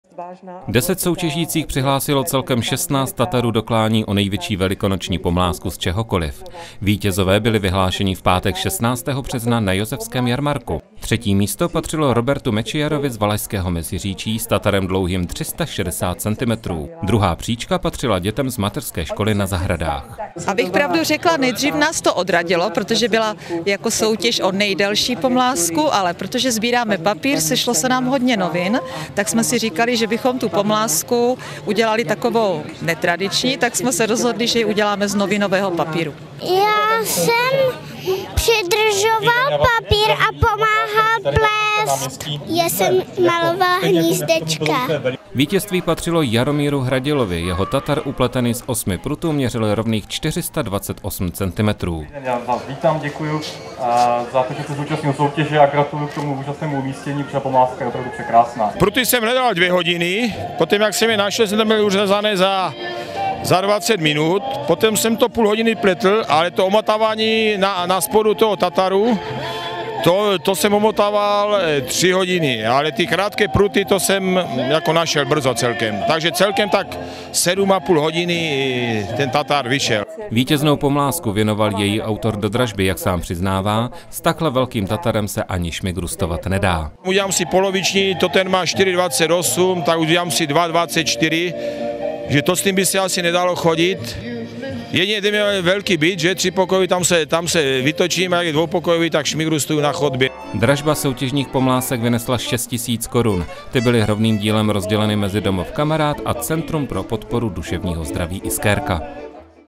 V Rožnově se soutěžilo o největší velikonoční pomlázku
Velikonoce na sebe sice ještě nechají pár dnů čekat, na Josefském jarmarku v Rožnově pod Radhoštěm byly ale už teď k vidění pomlázky. A to ne jen tak ledajaké. Ve zdejší soutěži se utkaly skutečně rekordní kousky. Ten nejdelší měřil více než 4 metry.